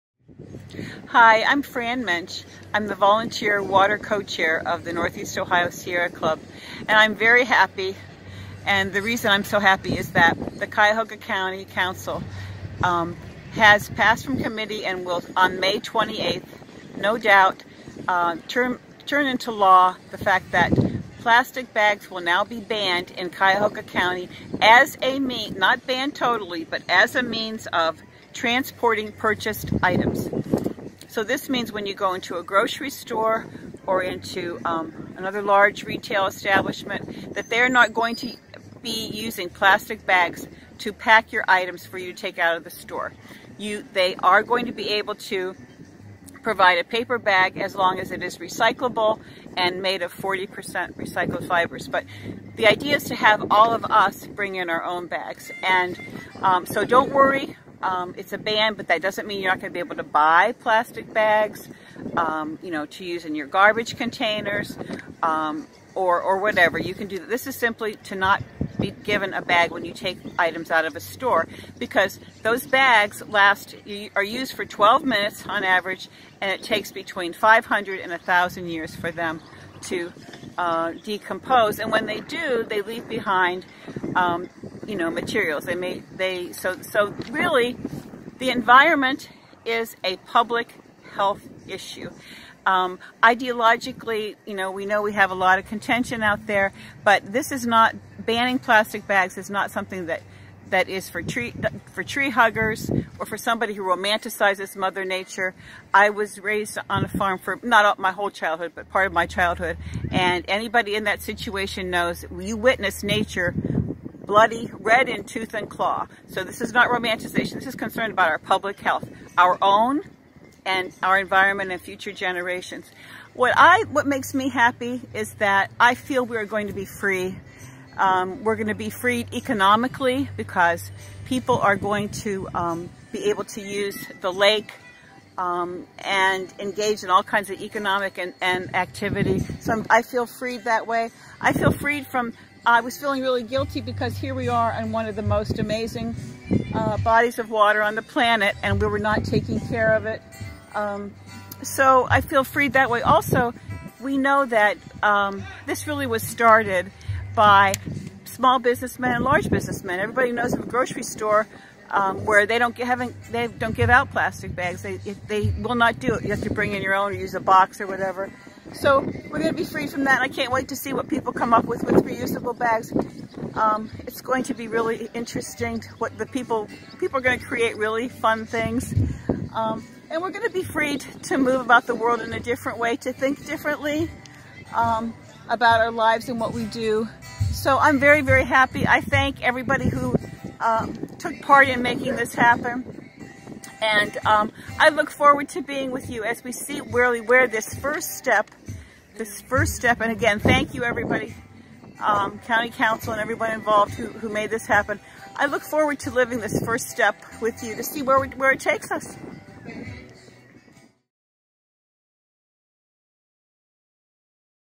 REPORT: Single Use Plastic Bags Cuyahoga County May 2019
REPORTING: Single Use Plastic Bags Cuyahoga County May 2019 with Fran Mentch, Volunteer Water Co-Chair, Northeast Ohio Sierra Club. On May 28, 2019 Cuyahoga County Council will likely turn into law a ban on single use plastic bags as a means to transport purchased items in Cuyahoga County. This action is to address environmental public health for us and future generations. The ban will encourage recyclable bag use. At Wendy Park, Cleveland, Ohio Saturday, May 11, 2019.